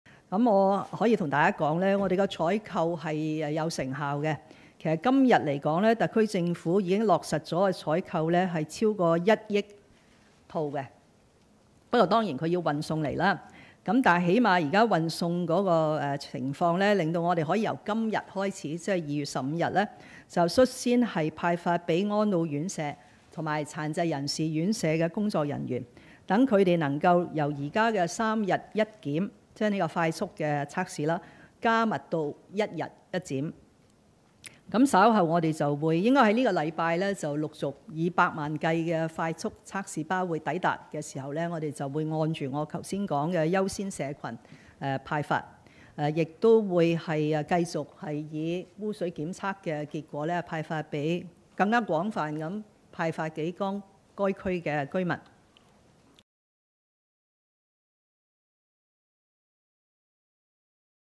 林鄭：已採購逾1億套快速測試包 日派100萬套
本港新冠肺炎疫情嚴峻，行政長官林鄭月娥今日（15日）出席行政會議前見記者時表示，快速抗原測試包採購有成效，已落實採購逾1億套，即日起陸續先向院舍工作人員派發。

林鄭表示，明白政府應對疫情未如理想，正不斷作出改善，包括本港自身病毒檢測力增加逾1倍。她說，已請求中央協助大幅增加檢測量，當局亦大量購買快速測試包，派發予高風險感染群組，如污水檢測呈陽性的區域，估計每日要動用100萬套，又指港府採購已有成效，目前已購買逾1億套，可率先派發給安老院舍員工，讓他們做到1日1檢。